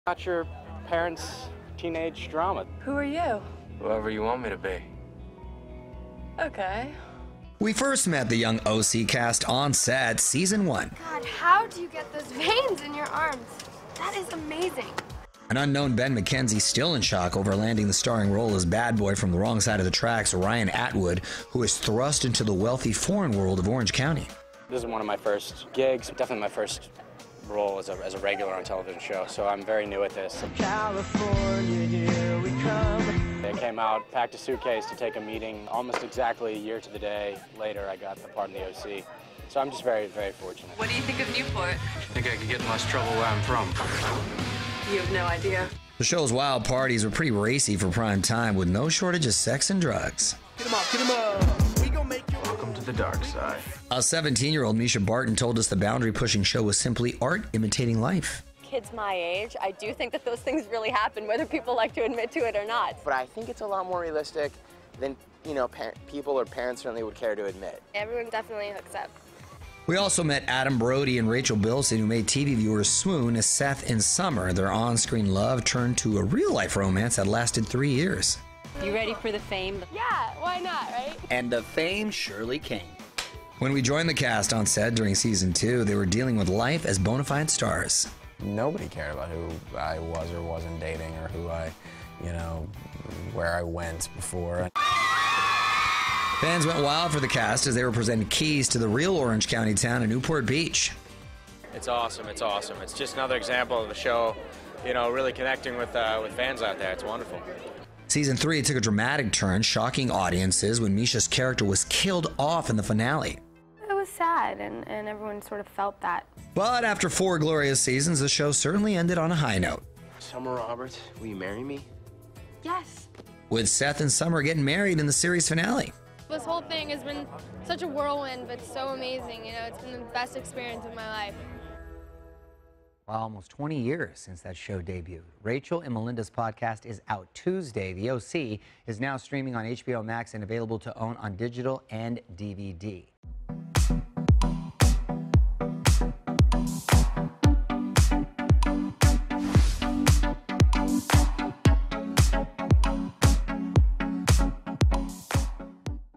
Looking Back At Mischa Barton, Adam Brody & Rachel Bilson On ‘The O.C.’ Nearly 20 Years Ago
We’re going back in time to a little place known as “The O.C.” Hit TV show, “The O.C.” first graced television back in 2003 with young stars Adam Brody, Rachel Bilson, Mischa Barton, and new-comer Ben McKenzie living the life of a typical teenager in Newport, CA. Access Hollywood was on the set of the first couple seasons, capturing behind the scenes footage and interviews with the cast in a close-up look of the sizzling hot show. Catch up on “The O.C.” now streaming on HBO Max and available to own on digital and DVD.